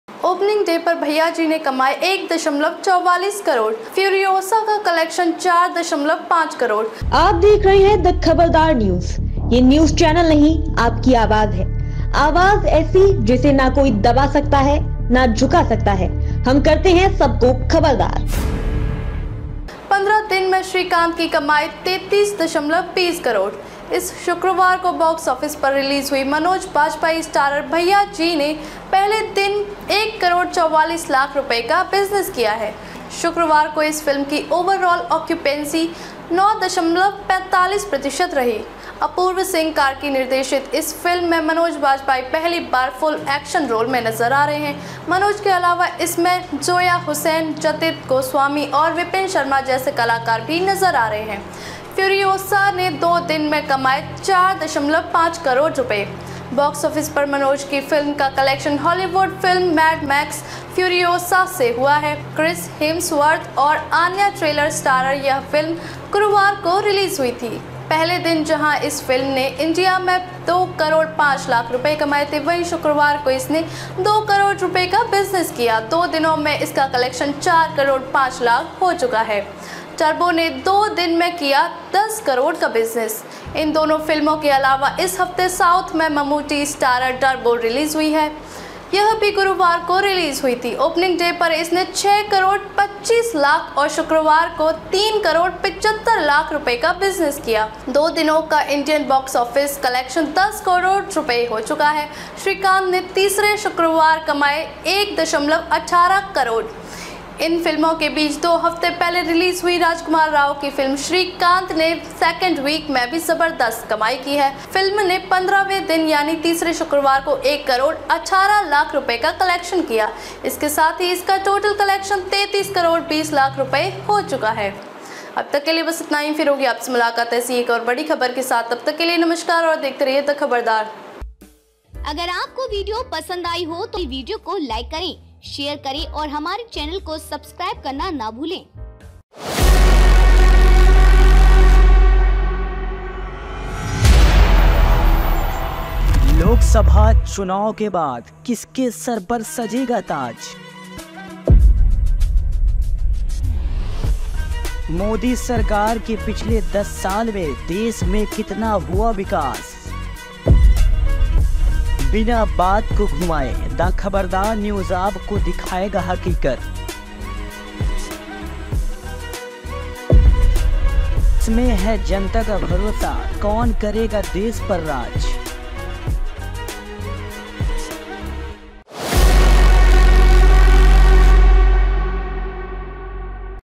Opening Day पर 'Bhaiya Ji' ने कमाए 1.44 करोड़, 'Furiosa' का collection 4.05 करोड़
शुक्रवार को इस फिल्म की ओवरऑल ऑक्यूपेंसी 9.45% रही। अपूर्व सिंह कार्की निर्देशित इस फिल्म में मनोज बाजपेयी पहली बार फुल एक्शन रोल में नजर आ रहे हैं। मनोज के अलावा इसमें जोया हुसैन, जतित गोस्वामी और विपिन शर्मा जैसे कलाकार भी नजर आ रहे हैं।